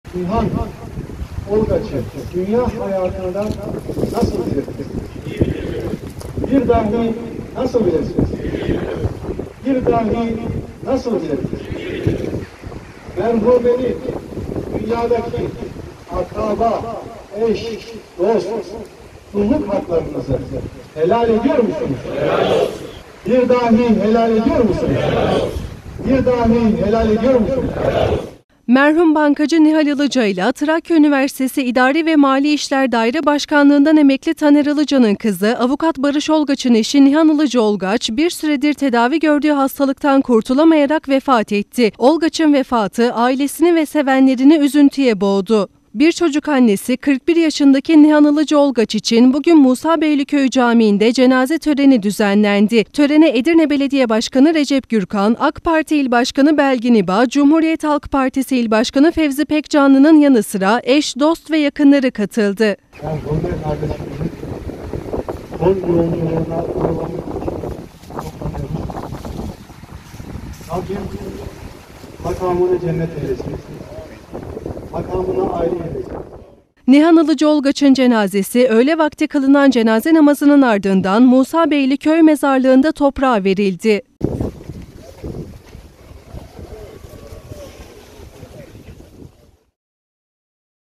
GENÇ YAŞTA YAŞAMA VEDA ETTİ
Edirne’de Avukat Barış Olgaç’ın eşi 41 yaşındaki Nihan Ilıca Olgaç, tedavi gördüğü hastanede vefat etti. Bir çocuk annesi Olgaç’ın vefatı, ailesi ve sevenlerini üzüntüye boğdu.